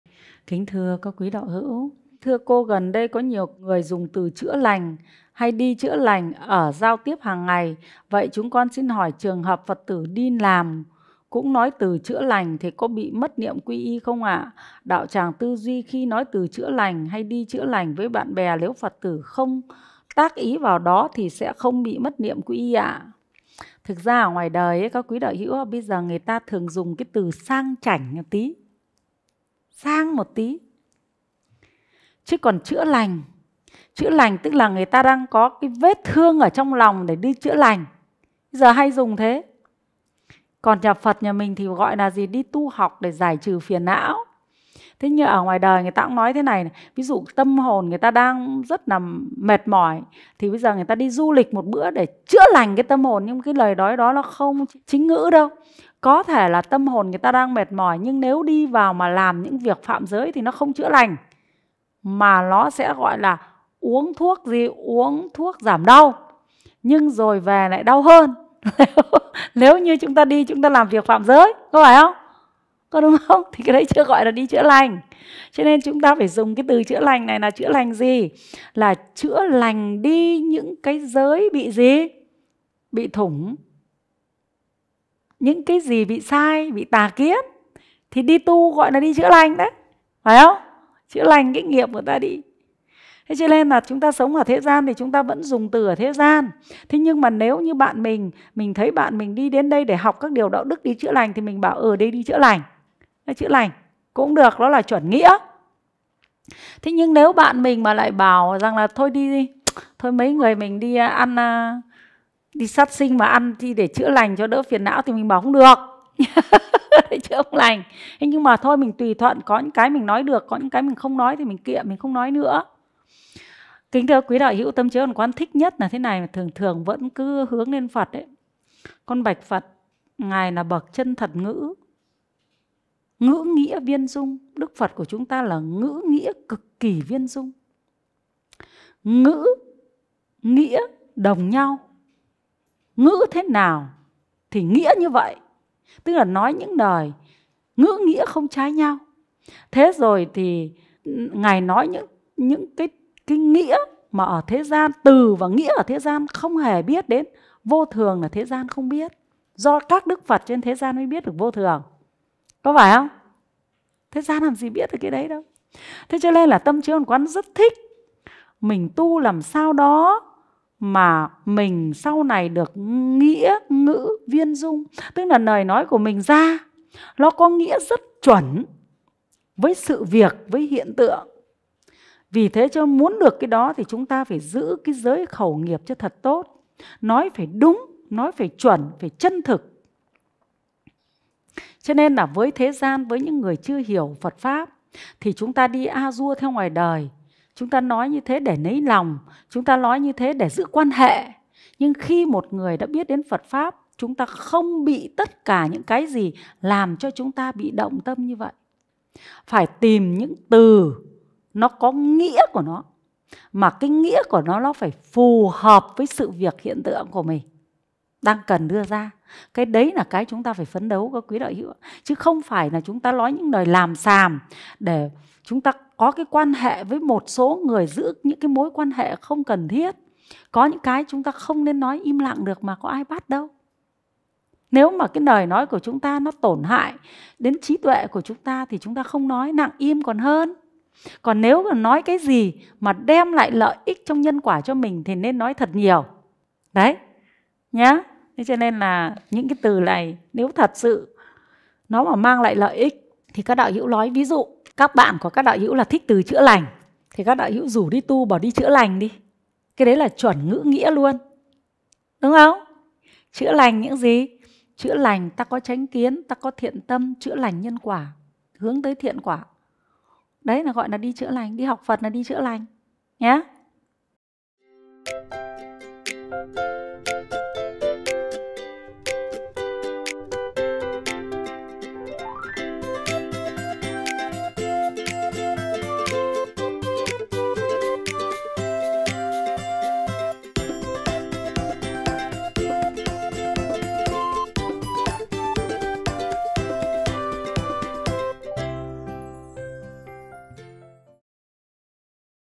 "Chữa lành" theo quan điểm Phật giáo
Cô Phạm Thị Yến - PD Tâm Chiếu Hoàn Quán chia sẻ với các Phật tử trong CLB.
Kính mời Quý đạo hữu xem thêm các bài theo các chủ đề khác:
Kính mời Quý đạo hữu tham gia các nhóm và các trang để được giải đáp thắc mắc: